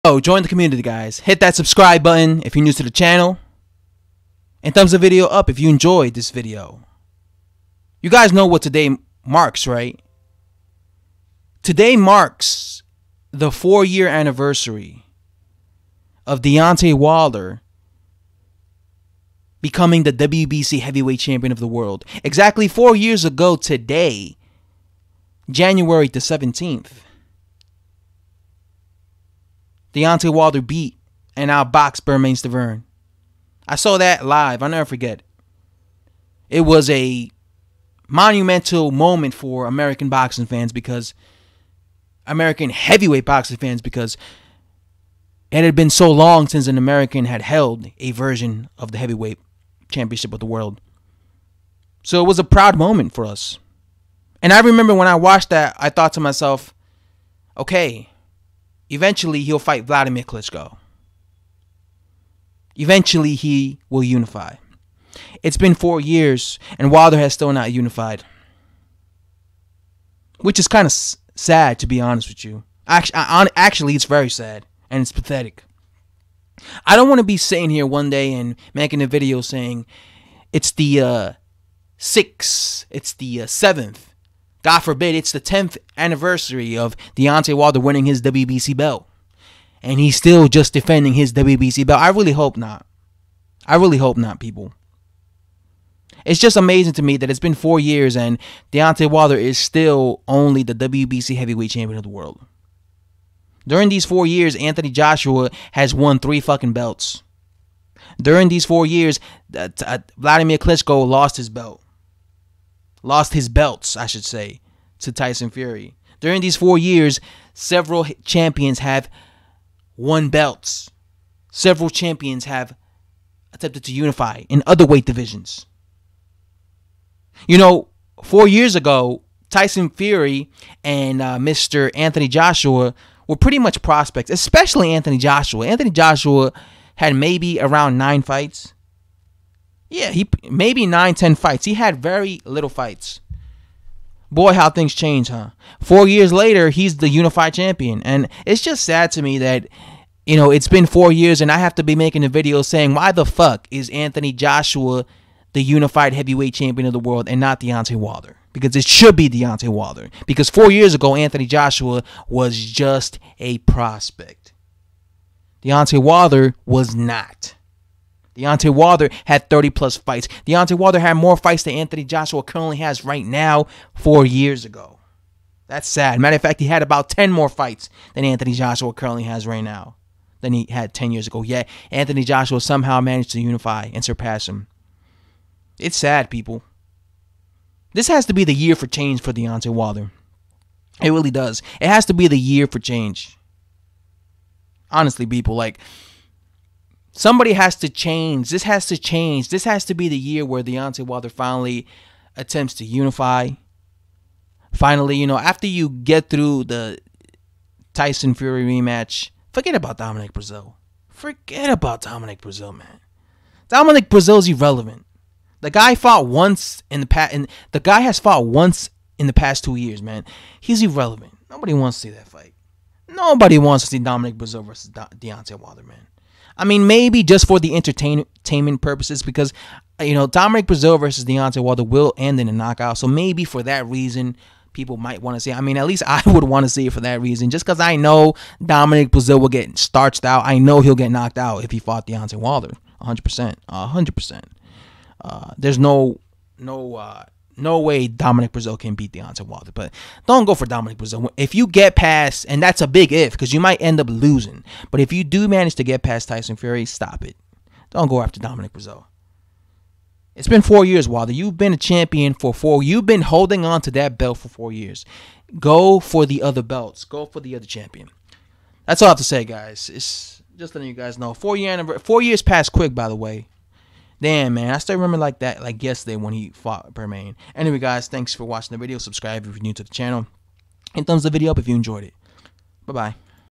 DEONTAY WILDER BECAME A CHAMPION 4 YEARS AGO ON THIS DAY
On Jan 15th 2015 Deontay Wilder Won The Wbc Heavyweight CHampionship of the world  4 years later He is STILL Not A Unified Champion